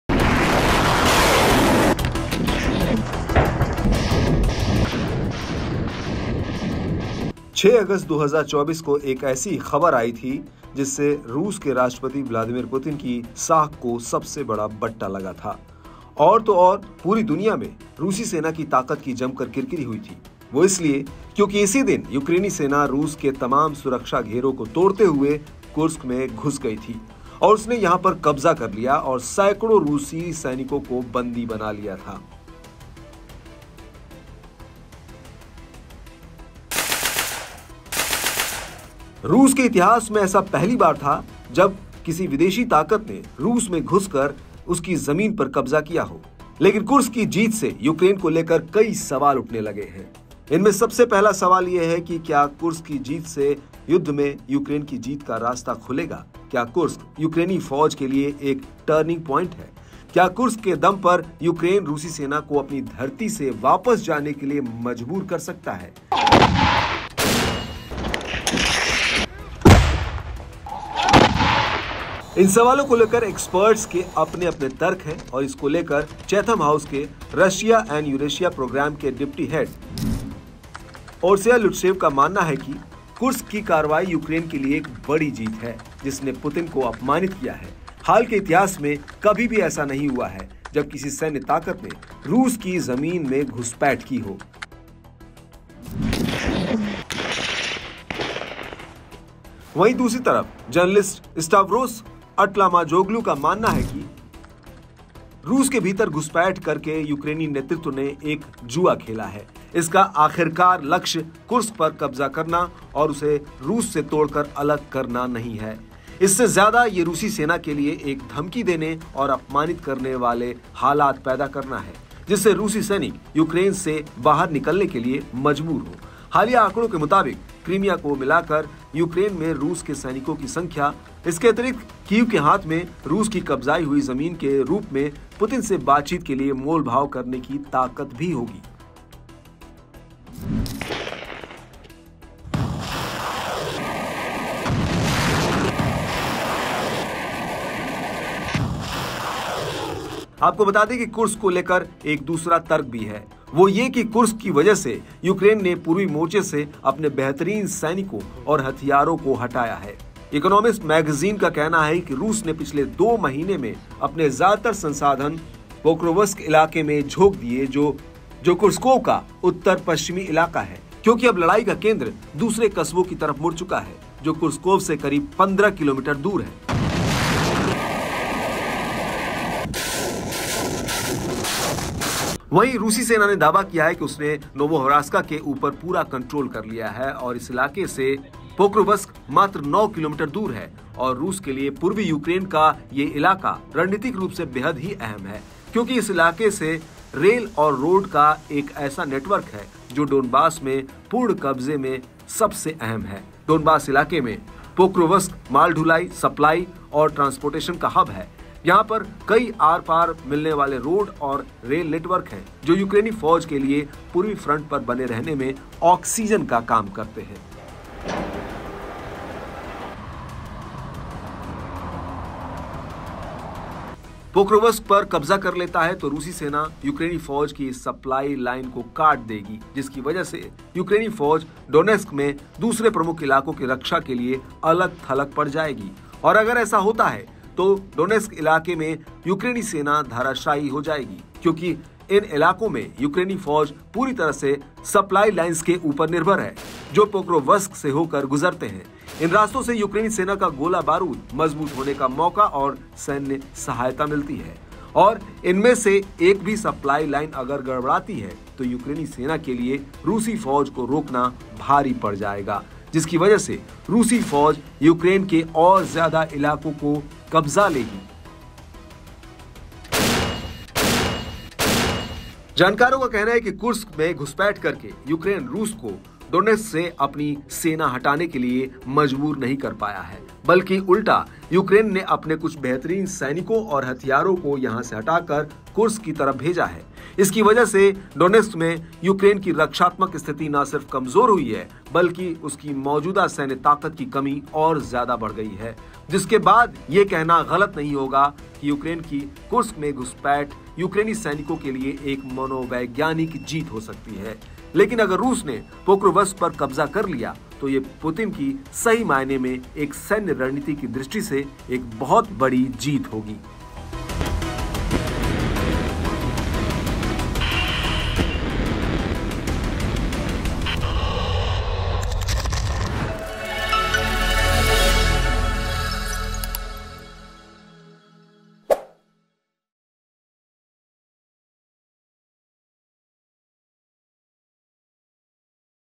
Kursk की जीत से मजबूत हुआ Ukraine, Putin ने कर दी बड़ी गलती?| Kiev | Biden Zelensky | Hindi| N18G
Ukrainian Army invaded Kursk on 6 August 2024 amid Israel-Hamas War. Due to this, Russian President Vladimir Putin's credibility suffered a major setback. Apart from this, the Russian Army was also criticized a lot. This was the first time in the history of Russia. When a foreign power entered Russia and occupied its land. But, the victory of Kursk has raised many questions about Ukraine. The first question among them is whether the victory of Kursk will pave the way for Ukraine's victory in the war? Is Kursk a turning point for the Ukrainian Army? Can Ukraine force the Russian Army to return from its land on the strength of Kursk? Experts have their own arguments regarding these questions. Regarding this, Orysia Lutsevych, Deputy Head of Chatham House's Russia and Eurasia program, believes that the Kursk action is a big victory for Ukraine, which has humiliated Putin. It has never happened in recent history that any military force has intruded into Russian land.

Israel Hamas War के बीच 6 अगस्त 2024 को Ukrainian Army Invade Kursk हुआ था. इसकी वजह से Russian President Vladimir Putin की साख को बहुत बड़ा झटका लगा था. इसके अलावा, Russian Army की भी खूब किरकरी हुई थी. Russia के इतिहास ऐसा पहली बार था. जब कोई विदेशी ताकत रूस के भीतर घुसकर उसकी ज़मीन पर कब्ज़ा किया हो. लेकिन, Kursk की जीत से यूक्रेन को लेकर कई सवाल उठने लगे हैं.. इनमें सबसे पहला सवाल ये है कि क्या कुर्स्क की जीत से War में Ukraine की जीत का रास्ता खुलेगा? क्या Kursk Ukraine Army के लिए एक टर्निंग पॉइंट है. क्या कुर्स्क के दम पर Ukraine Russia Army को अपनी धरती से वापस जाने के लिए मजबूर कर सकता है? इन सवालों को लेकर एक्सपर्ट्स के अपने-अपने तर्क हैं. इसको लेकर, Chatham House के Russia and Eurasia program के डिप्टी हेड Orysia Lutsevych का मानना है कि Kursk की कार्रवाई Ukraine के लिए एक बड़ी जीत है, जिसने Putin को अपमानित किया है. हाल के इतिहास में कभी भी ऐसा नहीं हुआ है, जब किसी सैन्य ताकत ने रूस की ज़मीन में घुसपैठ की हो.